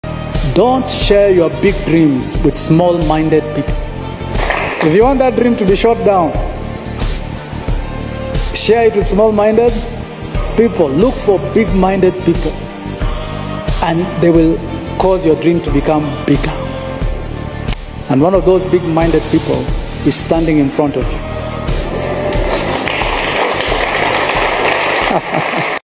Unlock Your Potential: Surround Yourself with Big-Minded People!
Discover the power of surrounding yourself with visionary individuals in this enlightening video.

We delve into the importance of protecting your dreams by avoiding small-minded influences and seeking out big-minded individuals who uplift, inspire and help you to unlock your full potential.

Learn how to unlock your full potential and why sharing your aspirations with small-minded people can stifle your growth and why connecting with big-minded individuals can amplify your vision.

Join us as we explore the transformative impact of aligning yourself with those who believe in the limitless potential of your dreams.

Prepare to be inspired and empowered as we unveil the secrets to expanding your dreams and achieving greatness. DreamBig VisionaryLeadership PersonalGrowth"